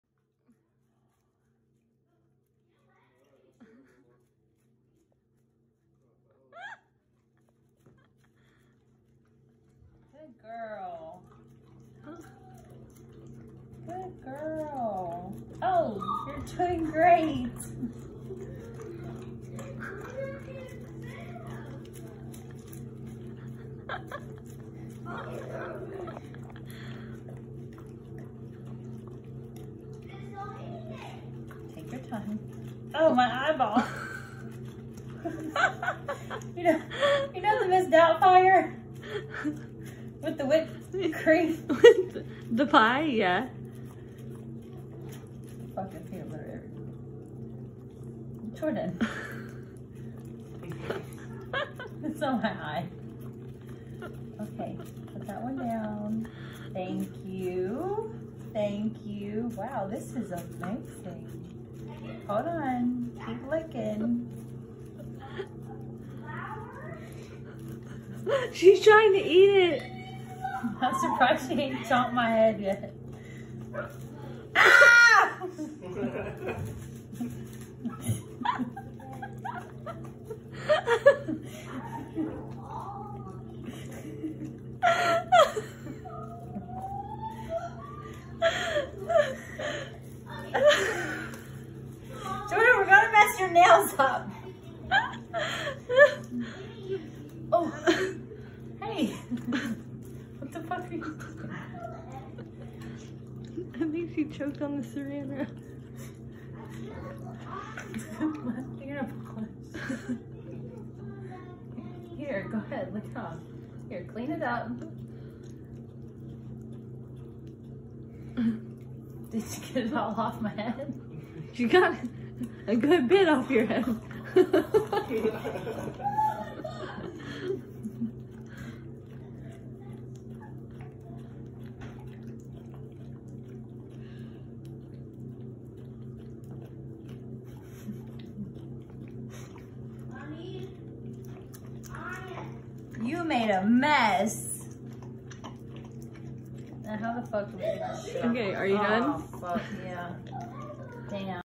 Sunday Funday shenanigans with the Goat Family.
Funday with our pups. Nail day for the lady.